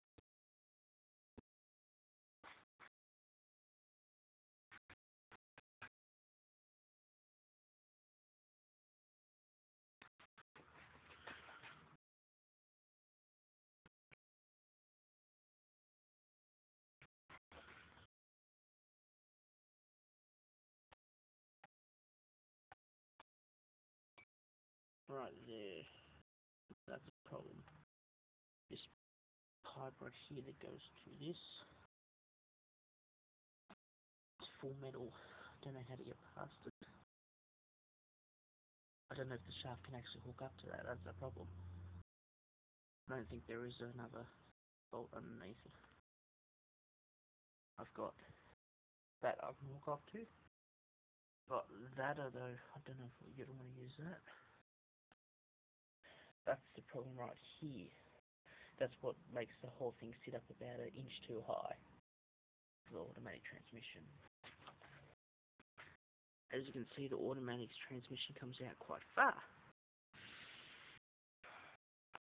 SC Fitment issue on ATX
trying to fit a MTX sc kit on a ATX Probe